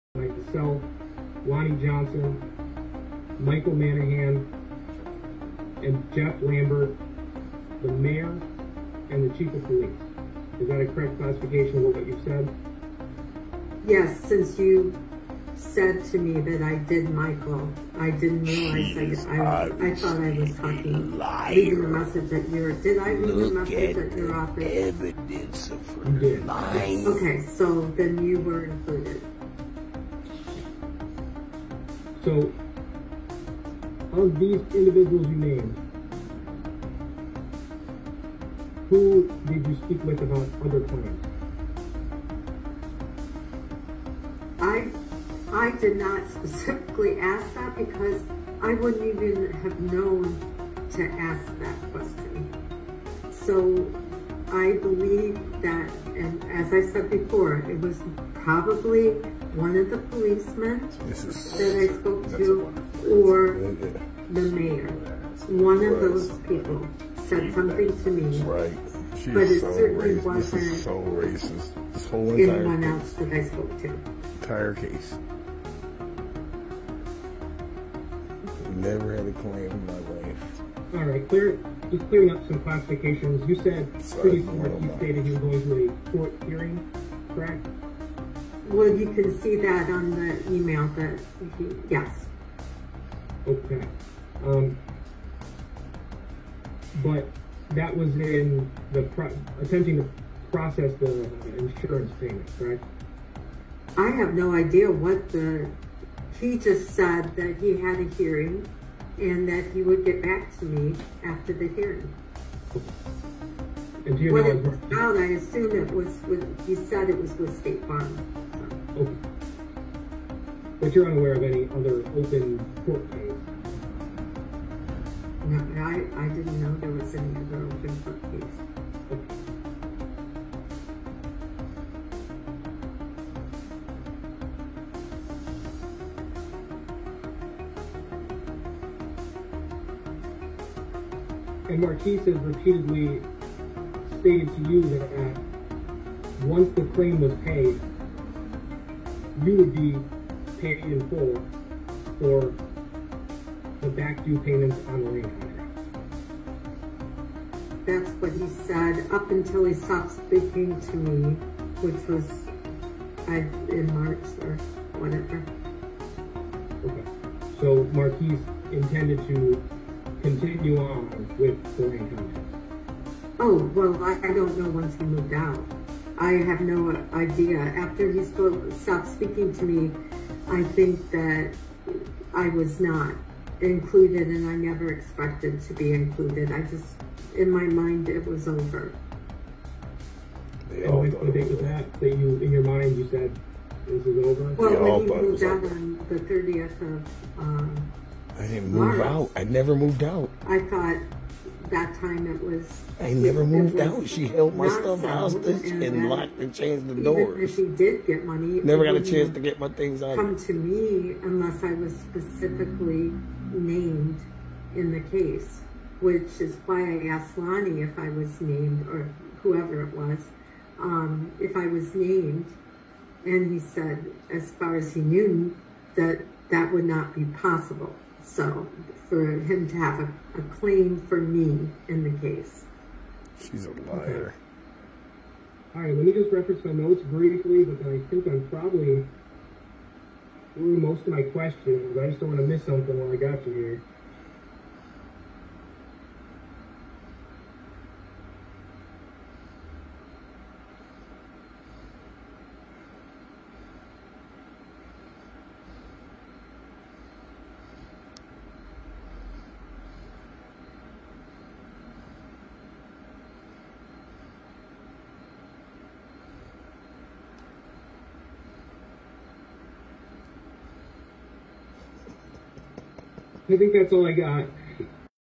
🔥State Farm Exposed: Suspect Confesses To State Farm Violating African American Business Rights🤔🔥
Suspect Confesses: State Farm Violated African American Business Rights!
SUSPECT ADMITS STATE FARM VIOLATED AFRICAN AMERICAN BUSINESS RIGHTS UNDER OATH
This is a MUST WATCH actual robbery suspect ADMITS STATE FARM violated african american business privacy rights under oath

Watch this video Suspect admits that State Farm Fire and Casualty unlawfully disclosed policy information to her without authorization. This violates the privacy laws concerning proper insurance procedures and policies.